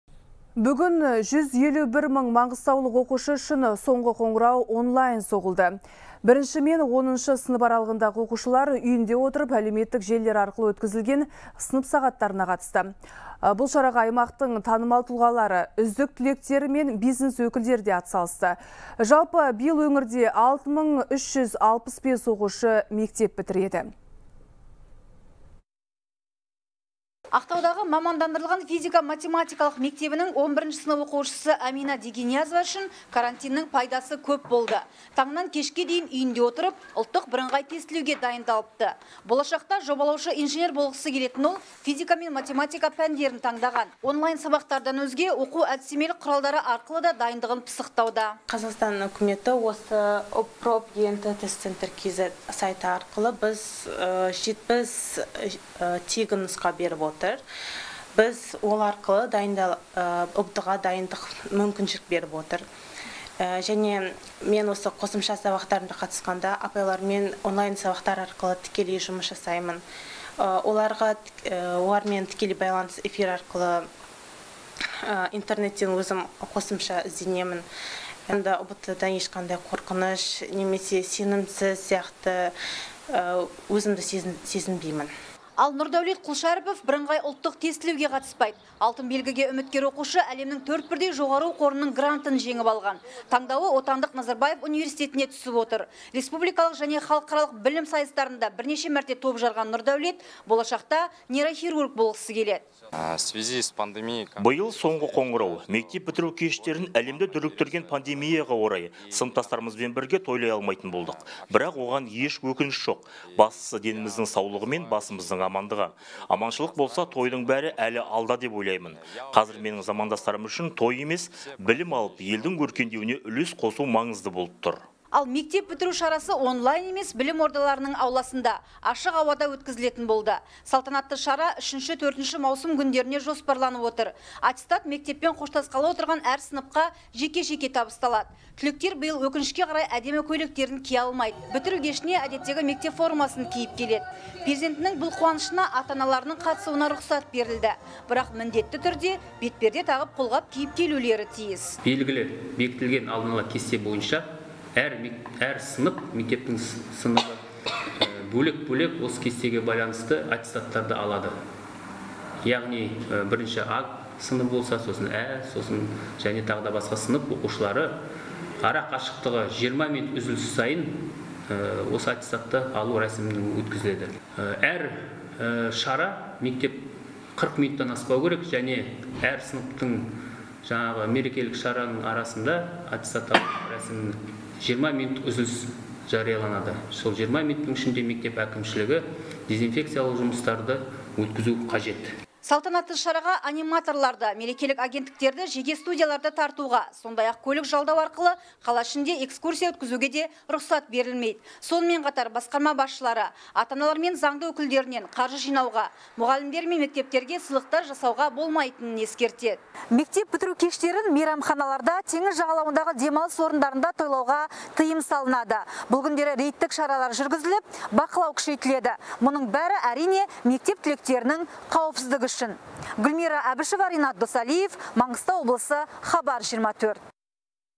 Маңғыстаулық оқушы әлемнің 4 бірдей жоғары оқу орнының грантын жеңіп алған
Бүгін 151 мың маңғыстаулық оқушы үшін соңғы қоңырау онлайн соғылды, деп хабарлайды «Хабар 24».